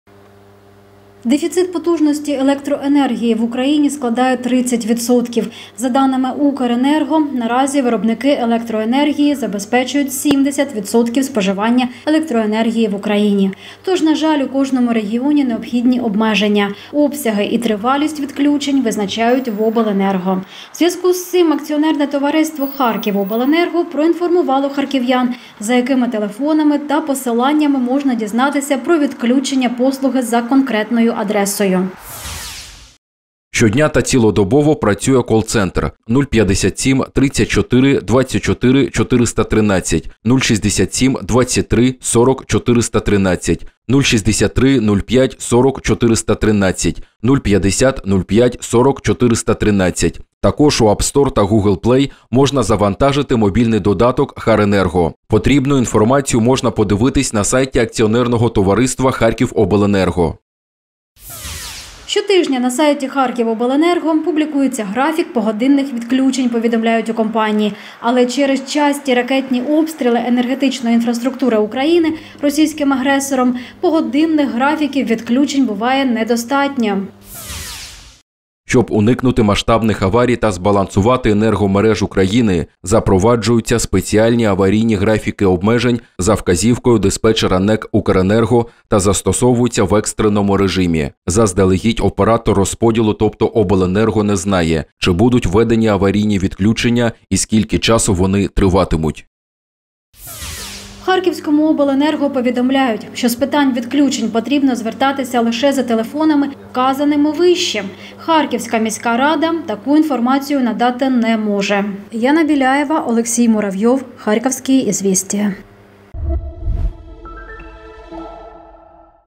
Аварійні відключення електроенергії у Харкові. Де дізнатись, куди телефонувати?
За даними "Укренерго", станом на ранок 29 листопада виробники електроенергії забезпечують 70% споживання електроенергії в Україні.
Відключення світла продовжуються по всіх областях, у кожному регіоні необхідні обмеження визначають обленерго. За якими телефонами дізнатися про відключення можуть харків'яни розповіли в АТ "ХАРКІВОБЛЕНЕРГО".